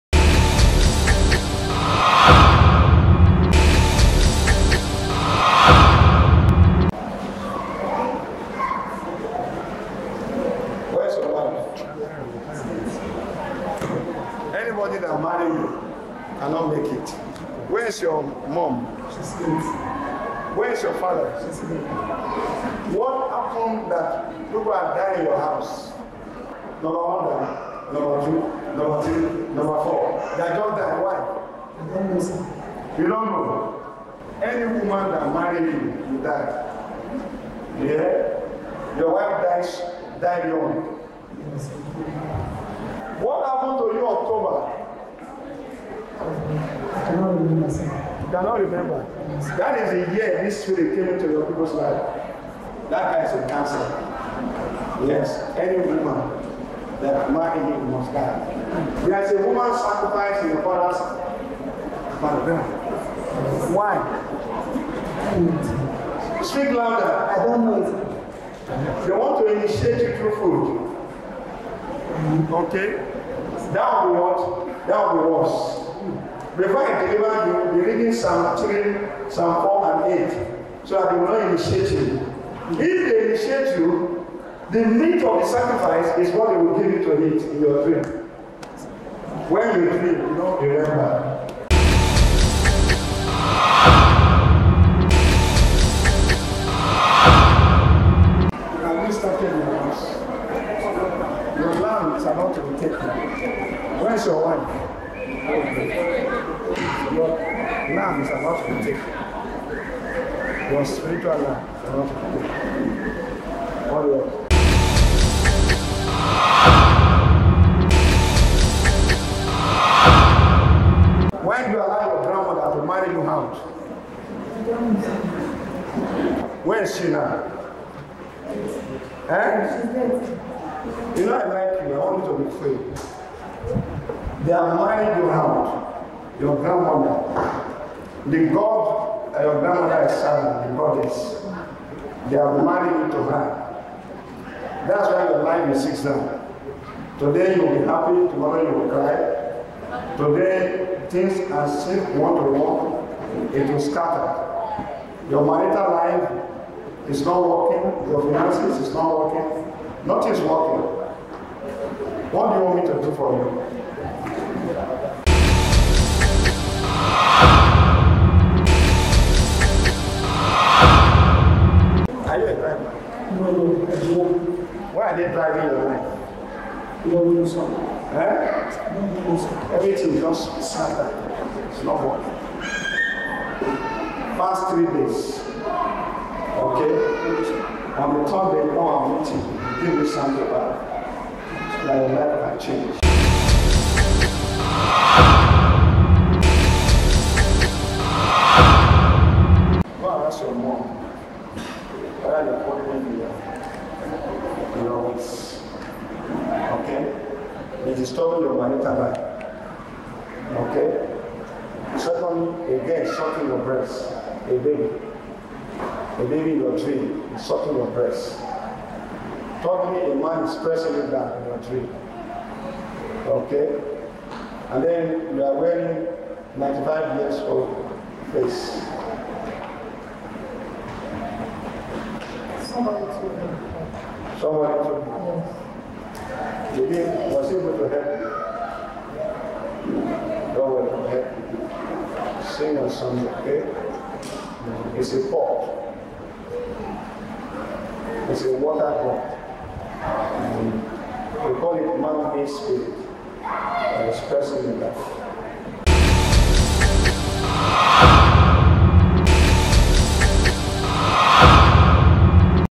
Anyone That Marries You Will Be Poor
See a man shocked to hear this from Prophet A. O. Omokoje during the Prophetic and Deliverance Service.